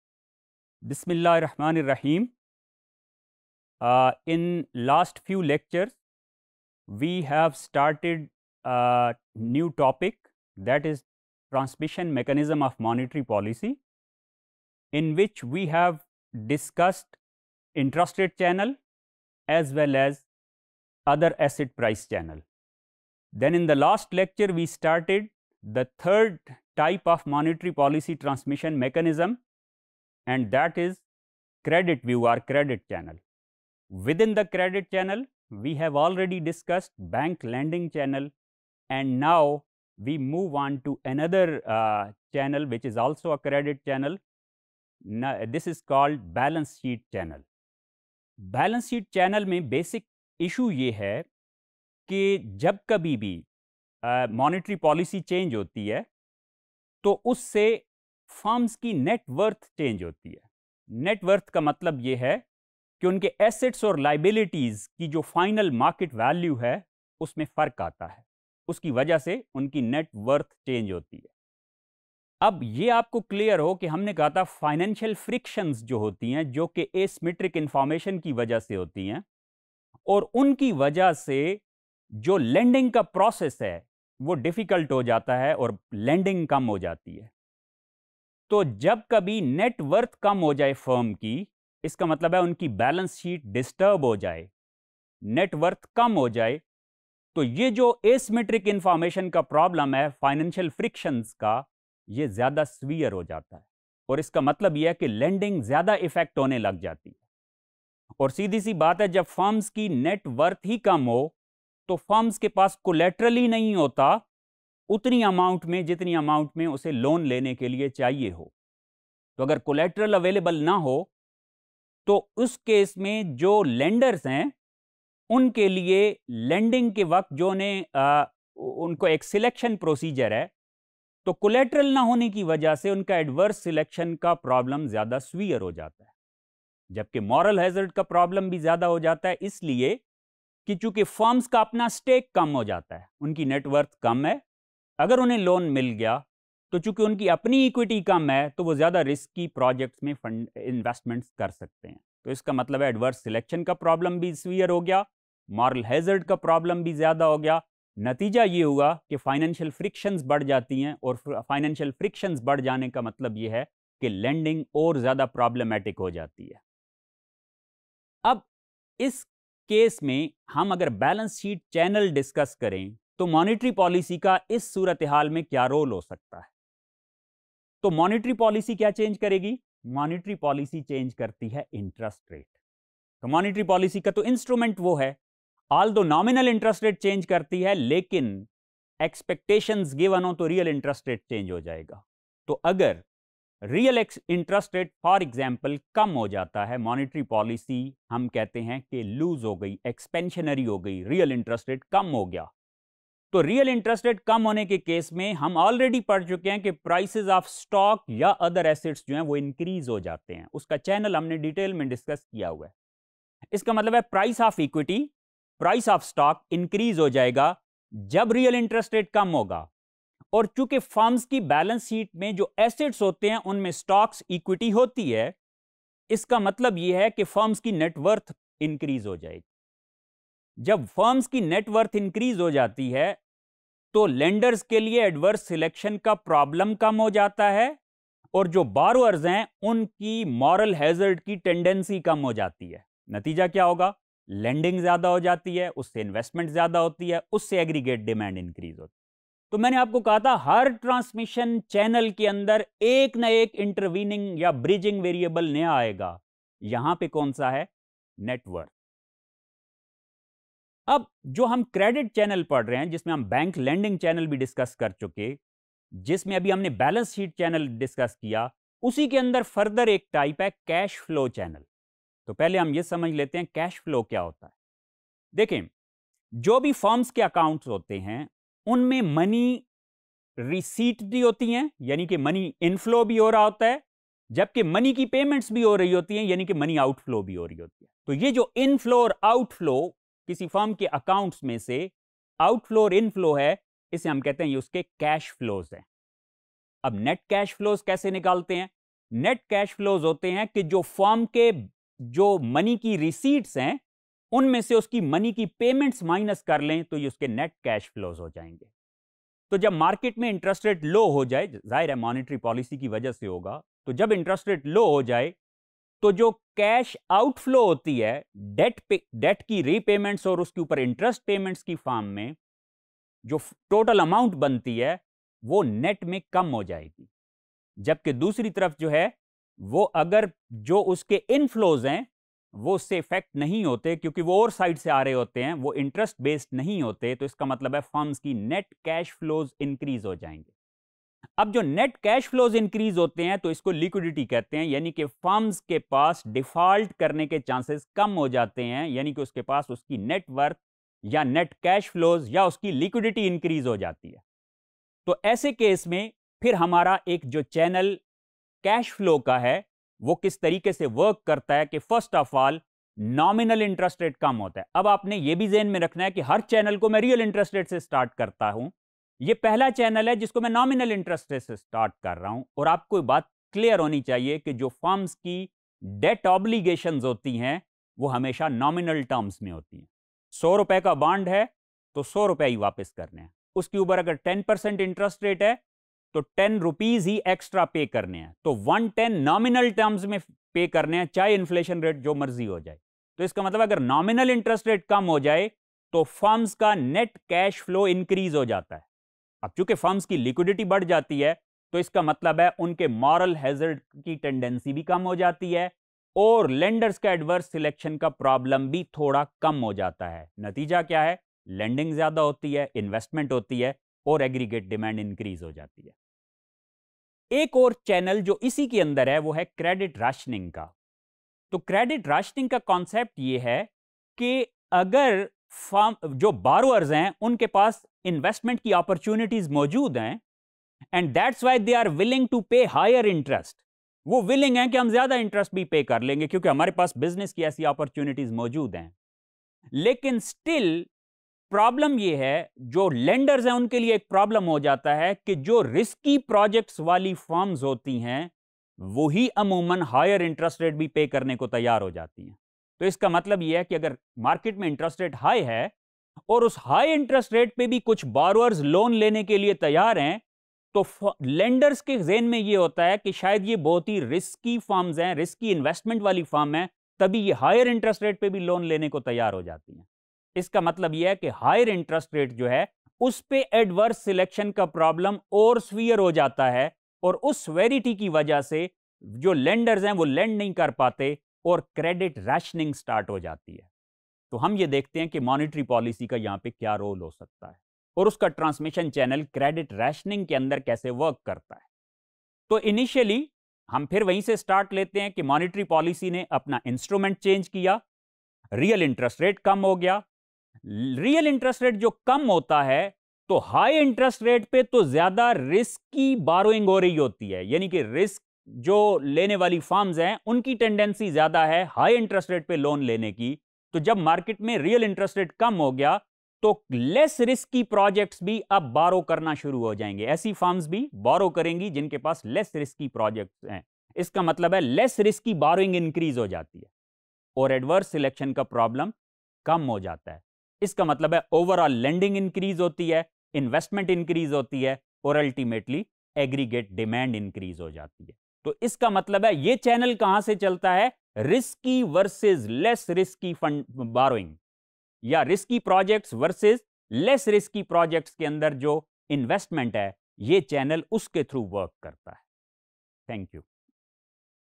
Transmission of Monetary Policy: Credit View, Balance Sheet Channel 1 | Economics | ECO604_Topic080
Topic080: Transmission of Monetary Policy - Credit View, Balance Sheet Channel - 1,
Meaning of Interest Rate,
By Dr. Wasim Shahid Malik